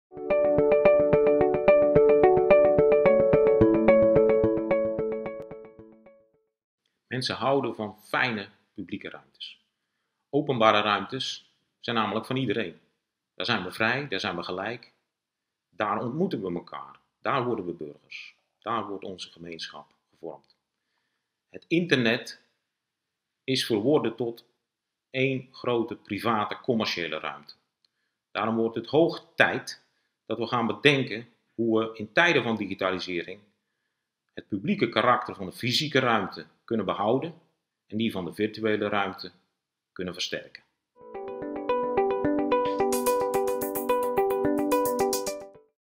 Rathenau Manifest - Ontwerpeis 10: we willen dat publieke ruimtes publiek blijven
De opkomst van AR, VR en spraaktechnologie vervaagt de grens tussen mens en computer, tussen fysiek en digitaal en tussen nep en echt.  Diverse publieke waarden zijn daardoor in het geding, waaronder privacy, autonomie en waarachtigheid. Daarom presenteert het Rathenau Instituut op basis van drie onderzoeksrapporten tien ontwerpeisen voor de digitale samenleving van morgen. Tien eisen die van overheden, bedrijven en burgers om urgente actie en dialoog vragen. Zodat immersieve technologie onze wereld rijker, en niet armer maakt.